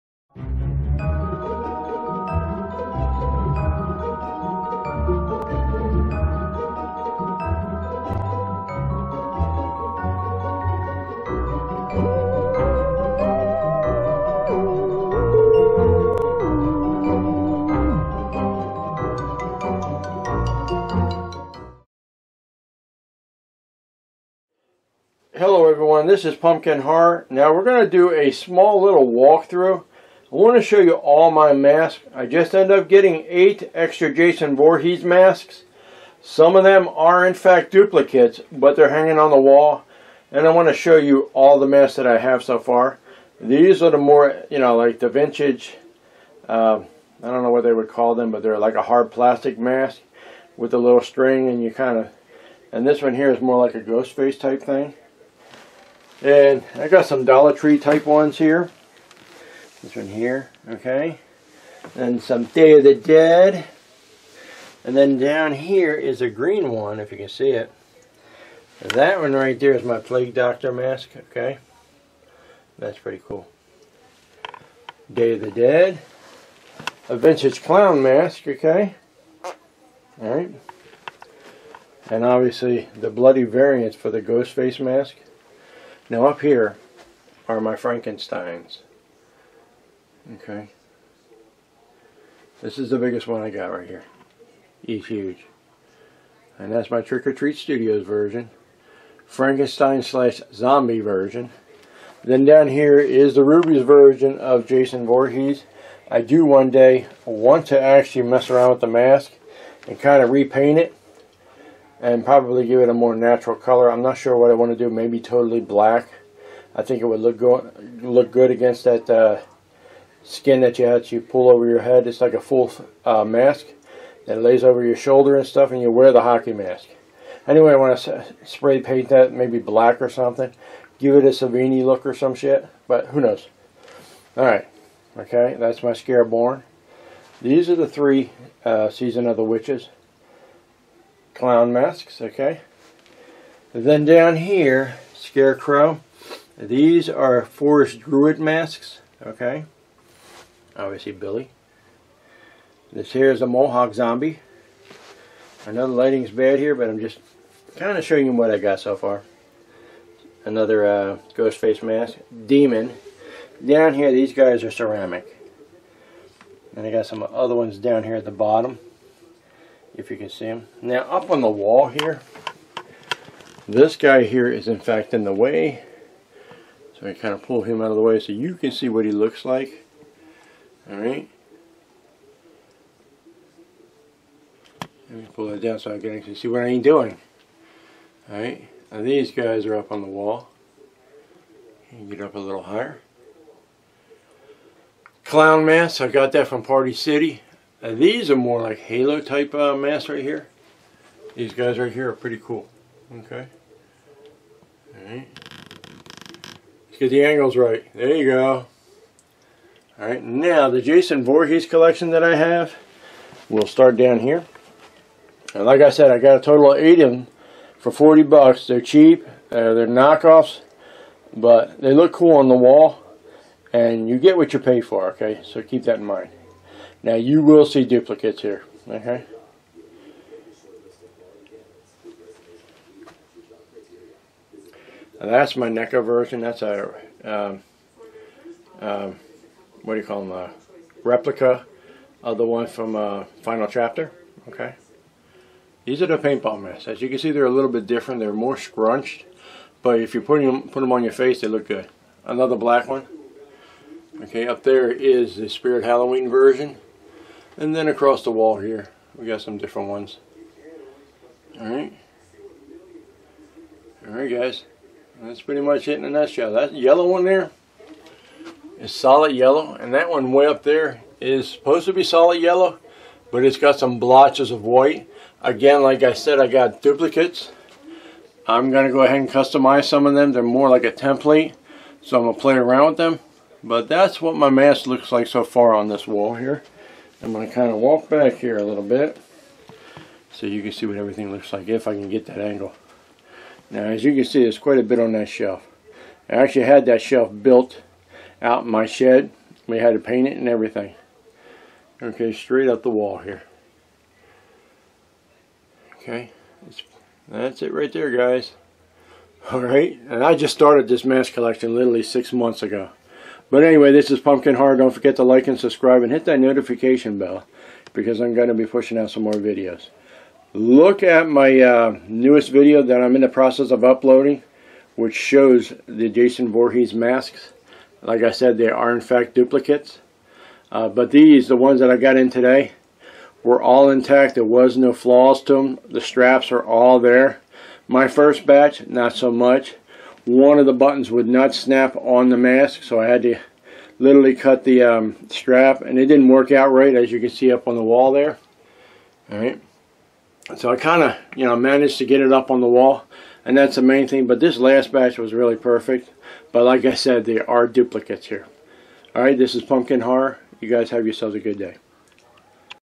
Mask Updated Walkthrough
Small walk through showing you all my updated mask collection!